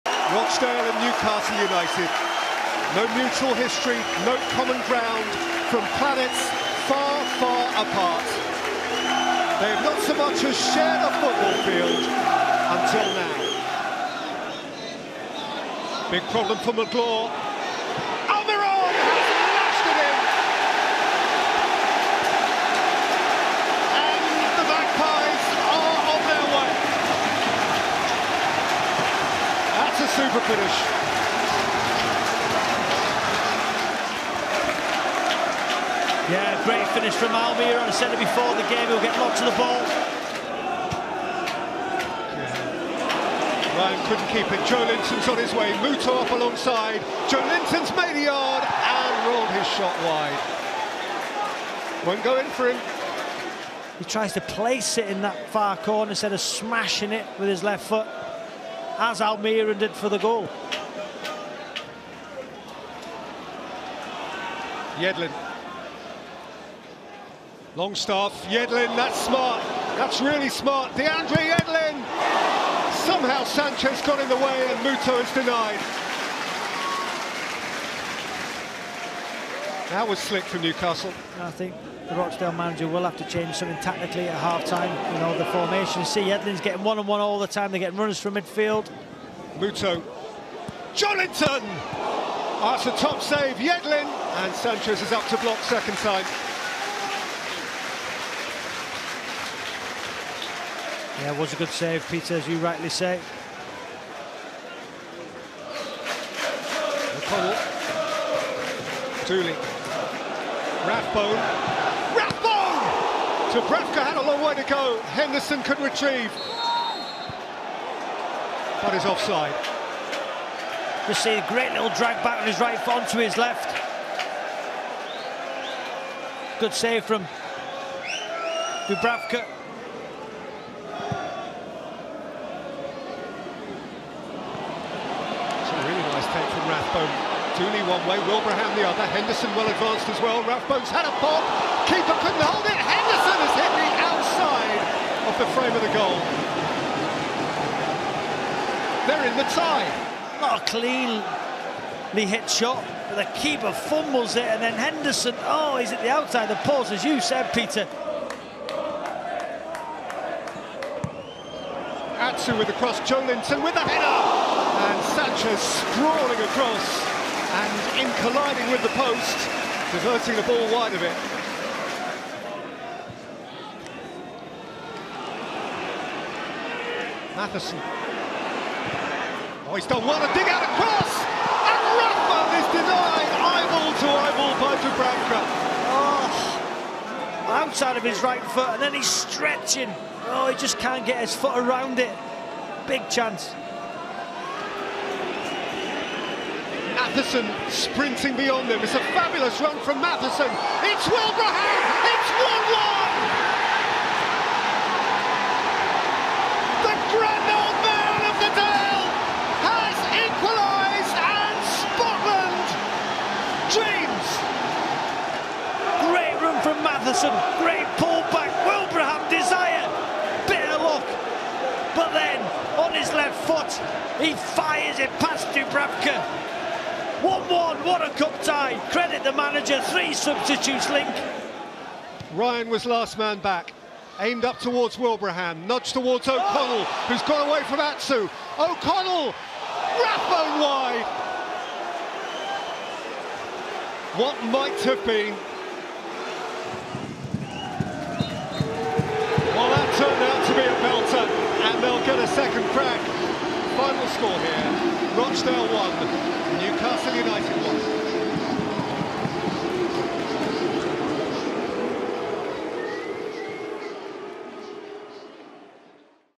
Rochdale v Newcastle (1-1) FA Cup Highlights
40-year-old Aaron Wilbraham ensures that Rochdale get a replay against Newcastle United following Miguel Almiron's opener.